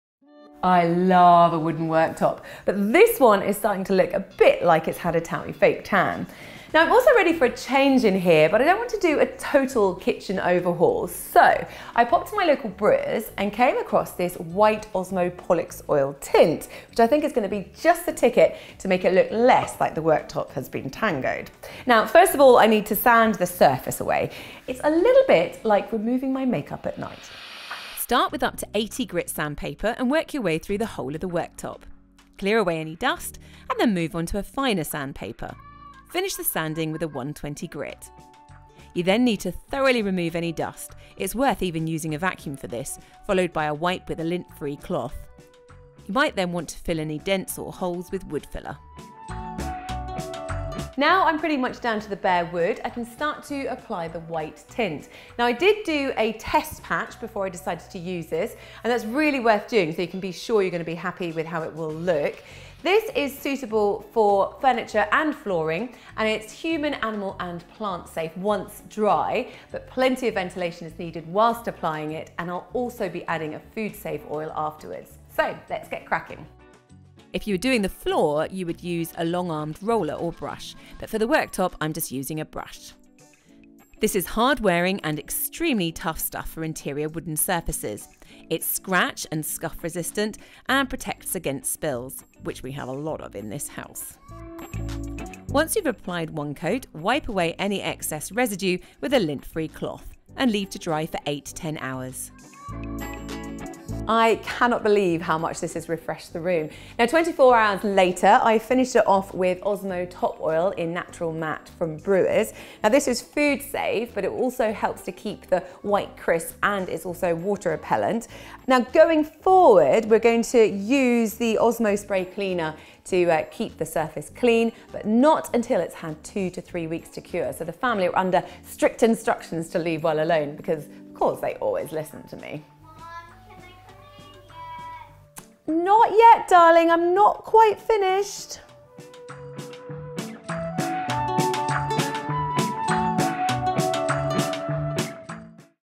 How To Renew Your Wooden Kitchen Worktop
Georgina shows you how to renew your kitchen top, step-by-step.